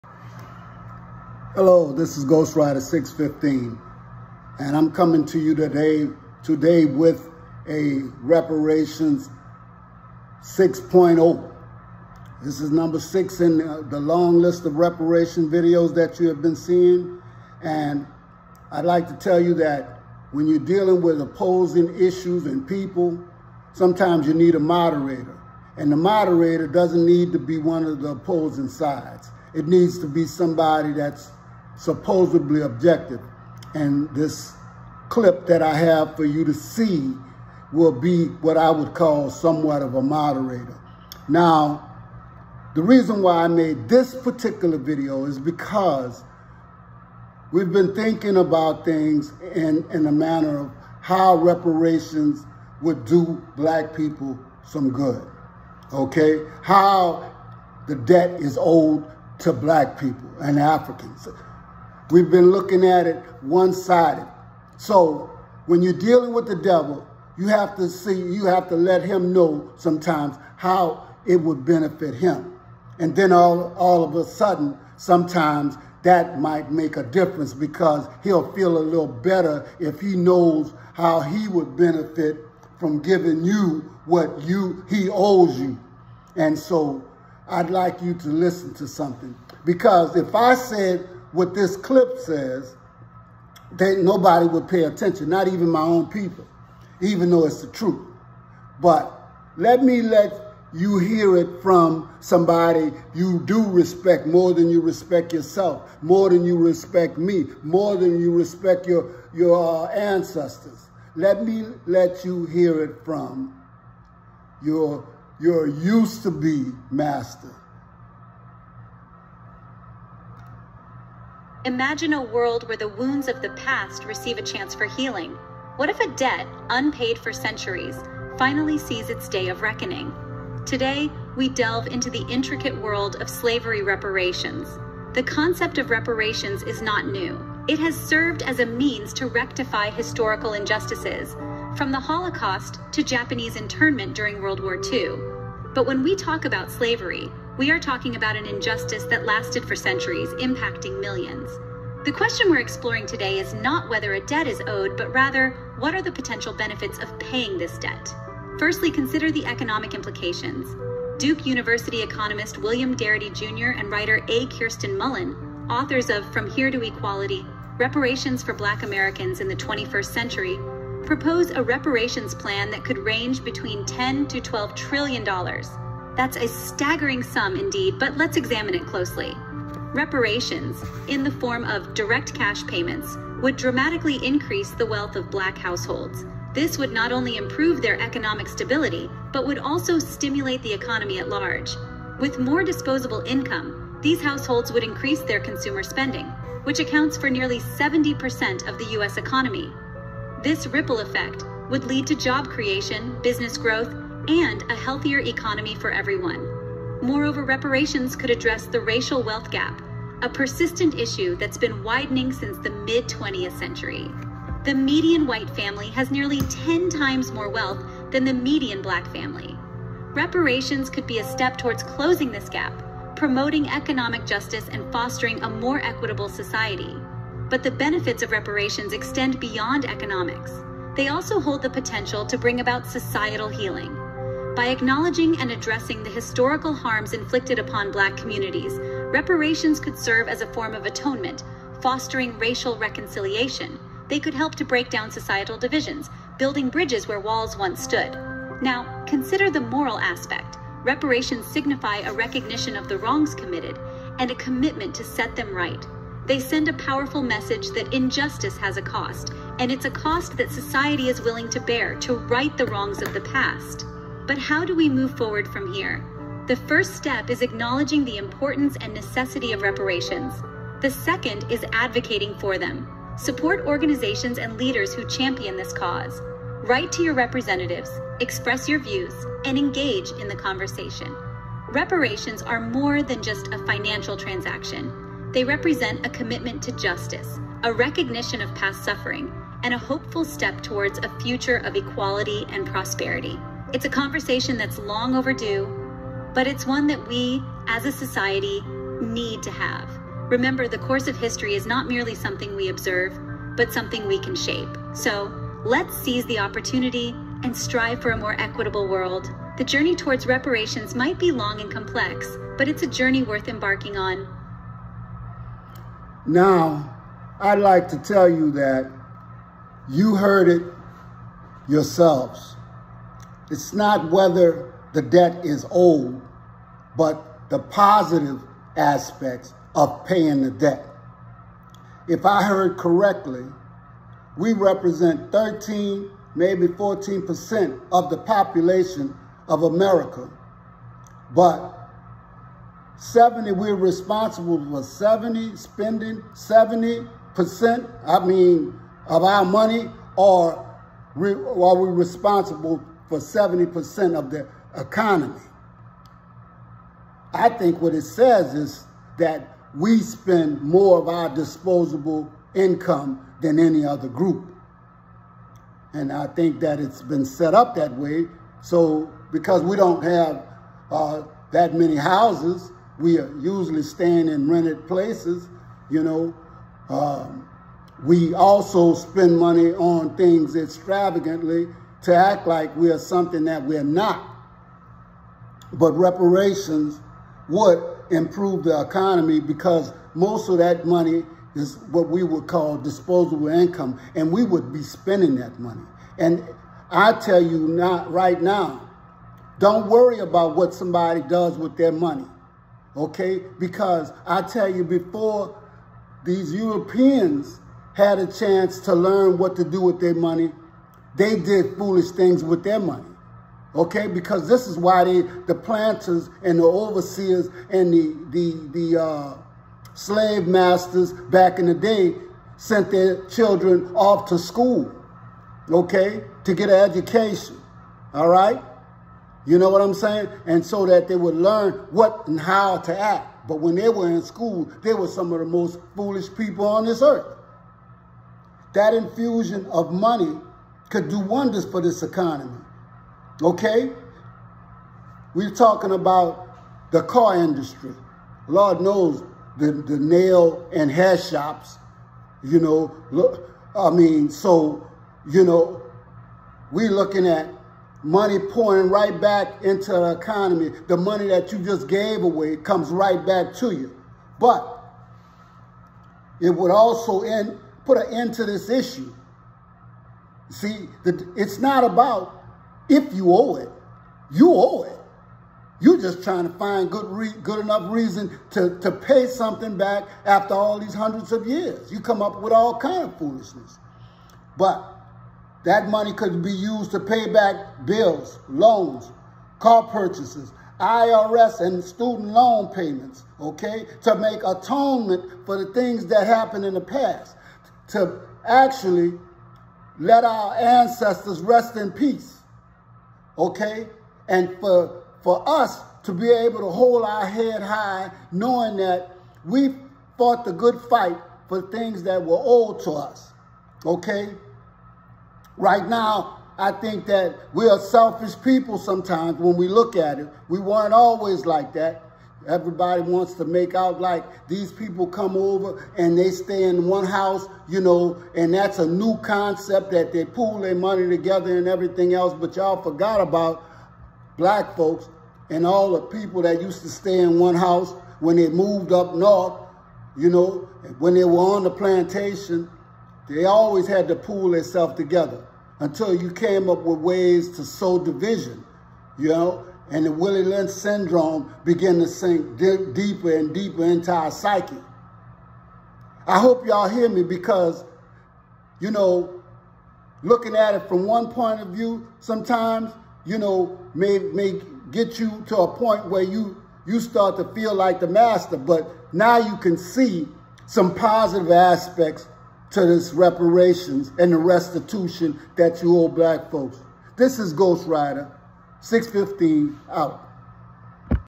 REPARATIONS 6.0
THE POSITIVE IMPACT OF SLAVERY REPARATIONS AND RESTITUTION FOR PAST CRIMES AGAINST HUMANITY. THE FIRST HUMANS ACCORDING TO SCIENTIFIC RESEARCH AND DATA. THE WORLD LEADERS AND PEOPLE IN POWER HAVE TRIED TO DEHUMANIZE US BUT WE ARE STILL HERE. IF WE EVER LEARN TO STAND TOGETHER AGAIN, THERE WOULD BE NOTHING WE COULD NOT ACCOMPLISH!